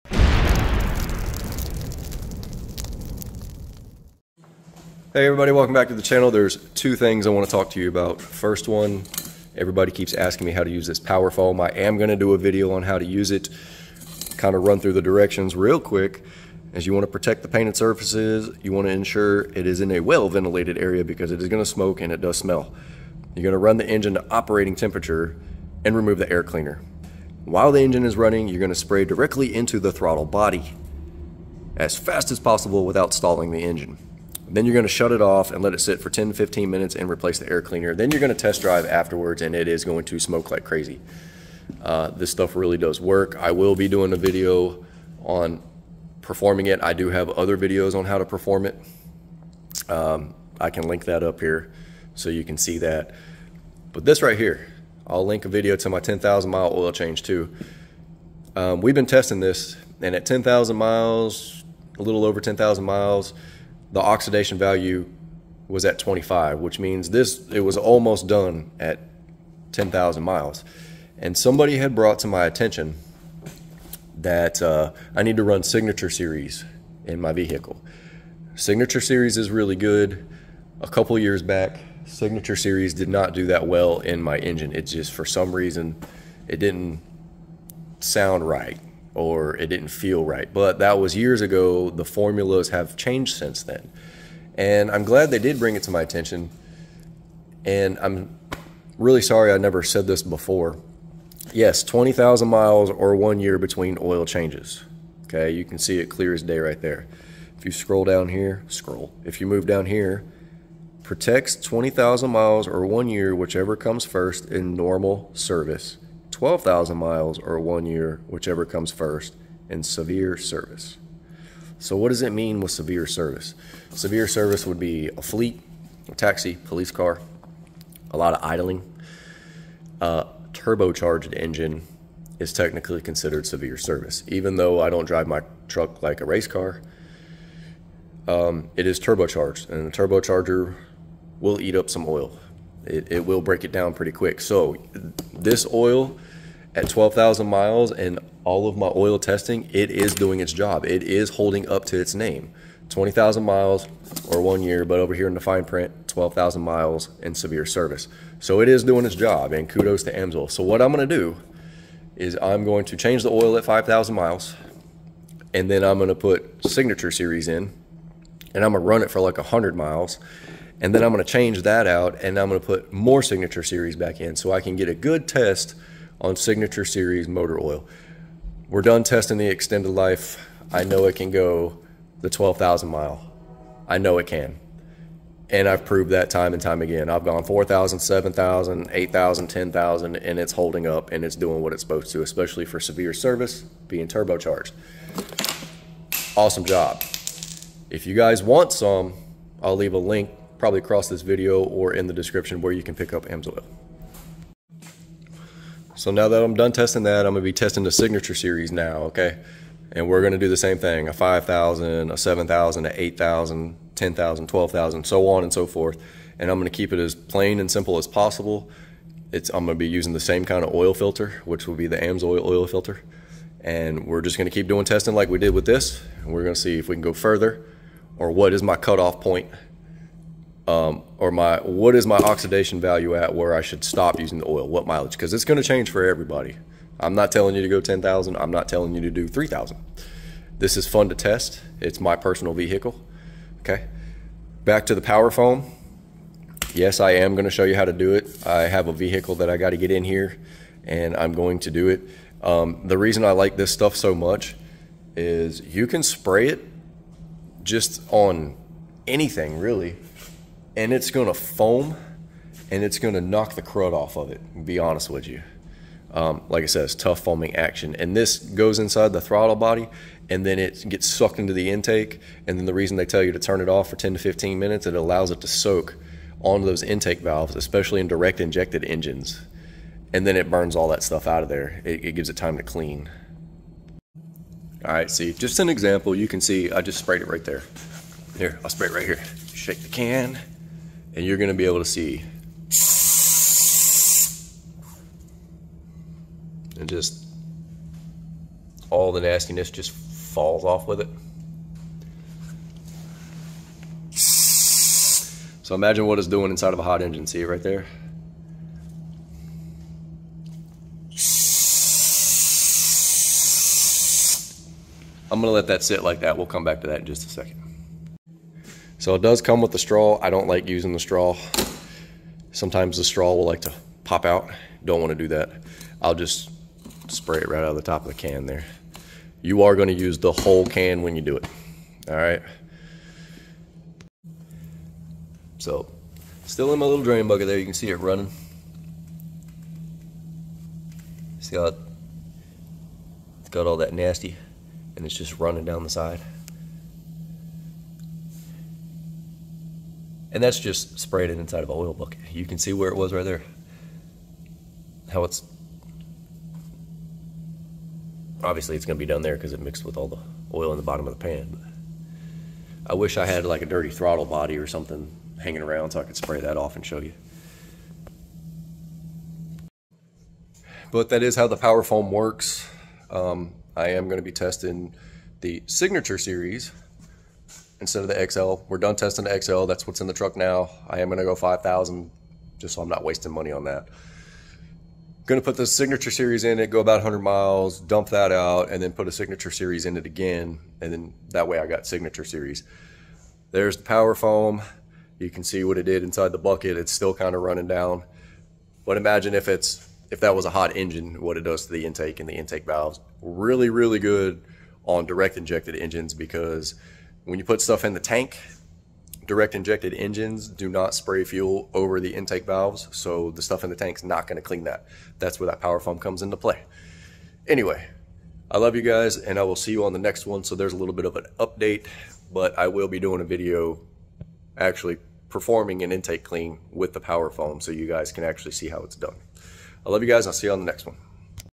Switching From Amsoil XL to Signature Series. What will Happen?
So I am going to be switching from Amsoil XL to Signature Series.  XL is good for 12k miles under severe duty. And a turbo charged engine is technically severe duty.  So Amsoil is living up to its claim.

And some brief instructions on Amsoil Power Foam.  Video Coming Soon.